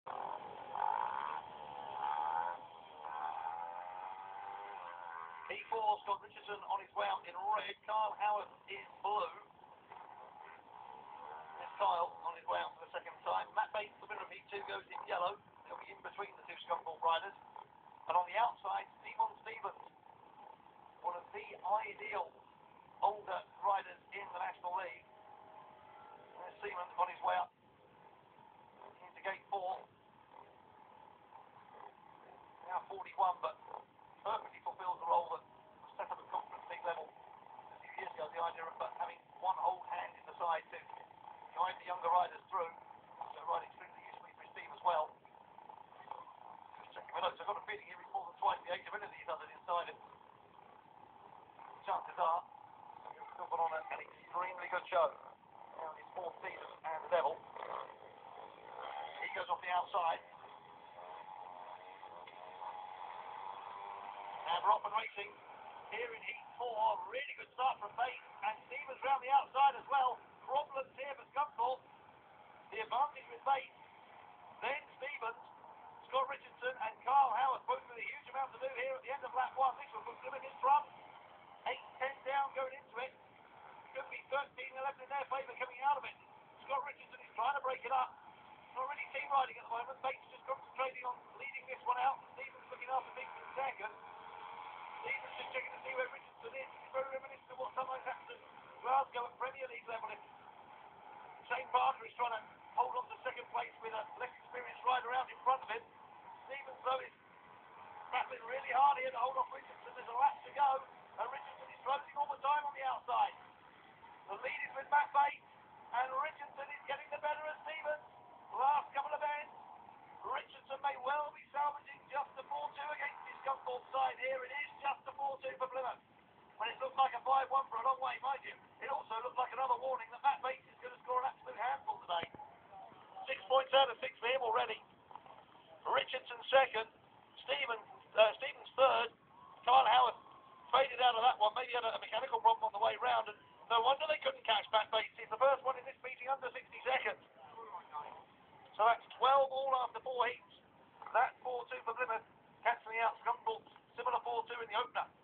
Racing at Scunthorpe
Scunthorpe: Scott Richardson (R), Kyle Howarth (B)
Plymouth: Matt Bates (G), Seemond Stephens (Y)  recorded of ma tv lastnight lol